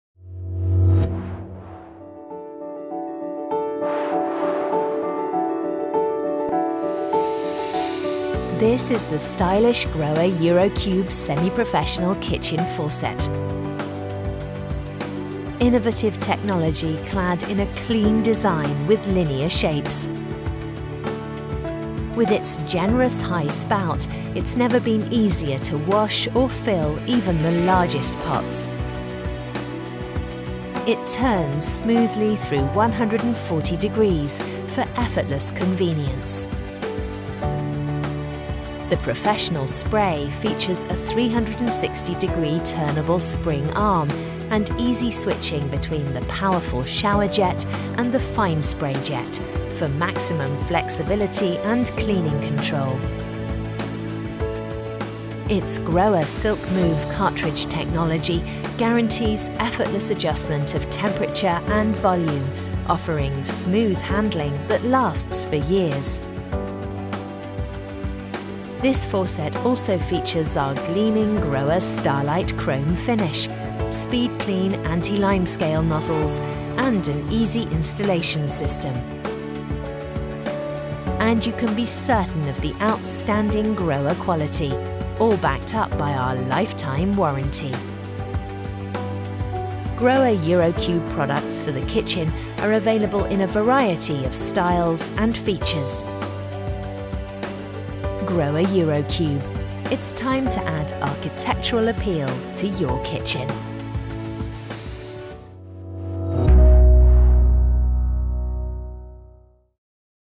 Полупрофессиональные кухонные смесители GROHE Eurocube
Полупрофессиональный смеситель Eurocube с выдвижной распылительной головкой был разработан как профессиональный инструмент для поваров, которым нравится комфорт и удобство на кухне. Устанавливаемая на пружинный шланг с перемещением на 360°, функция тумблера распыления позволяет плавно переключаться между обычным потоком воды и разбрызгиванием без отключения воды, обеспечивая максимальную гибкость и действительно профессиональную производительность.